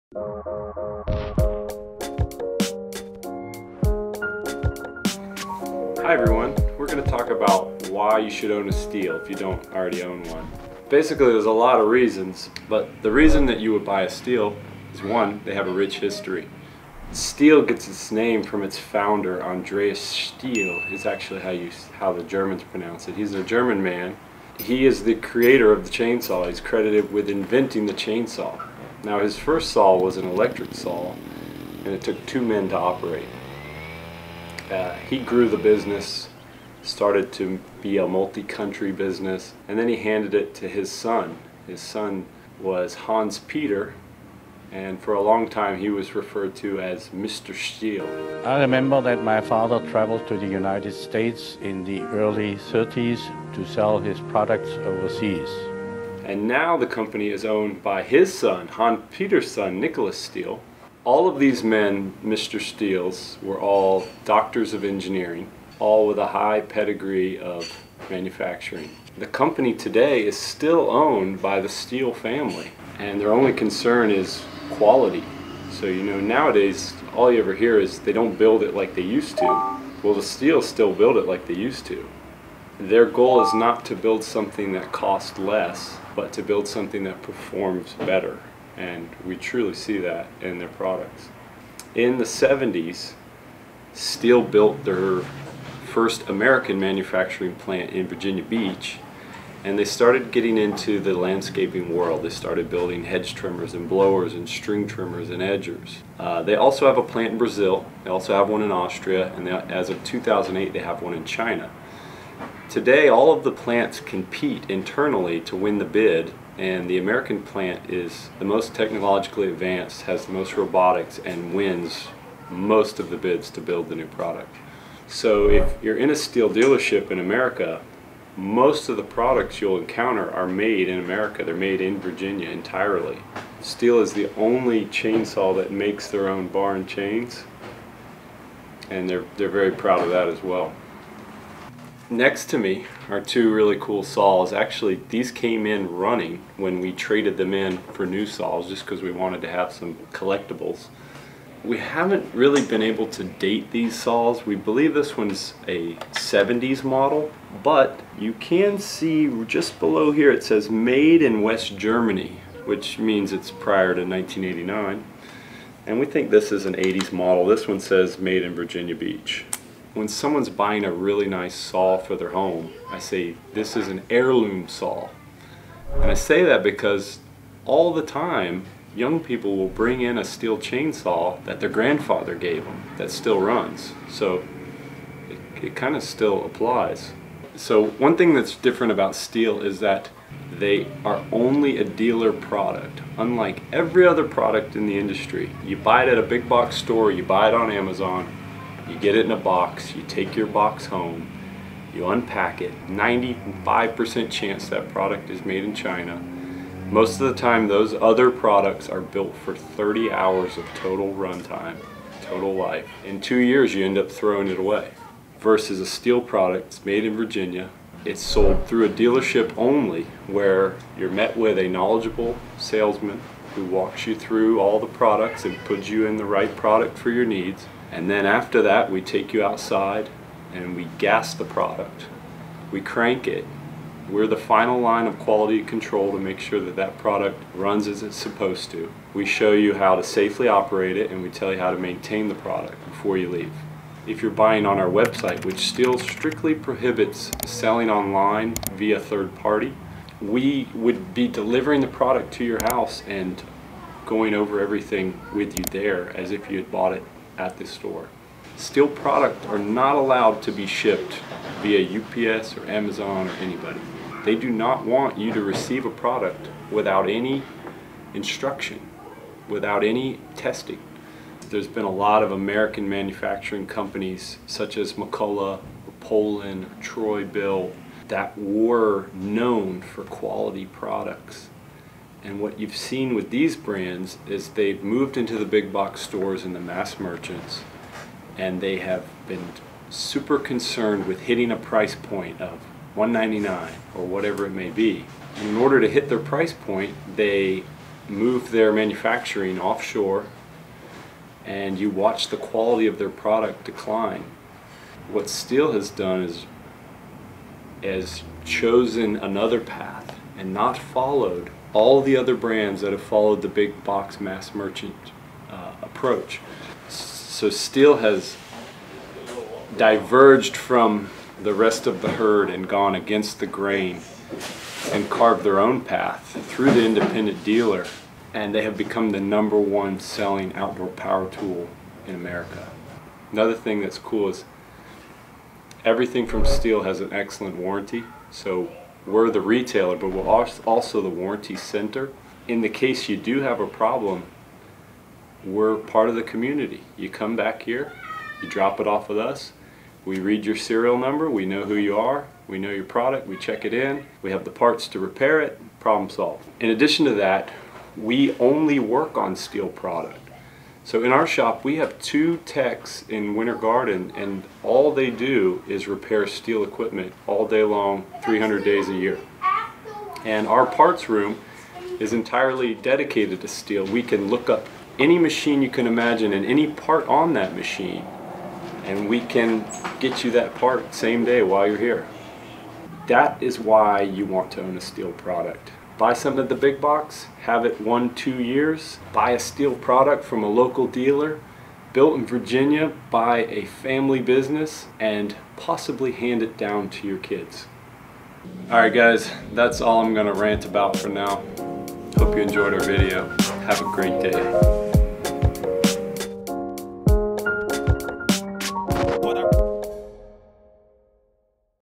Why Buy STIHL? Top 5 reason - Best Landscaping products 2020
Check out the TOP 5 reason why STIHL is the best outdoor power tools company in the market right now, and why you should absolutely own a unit!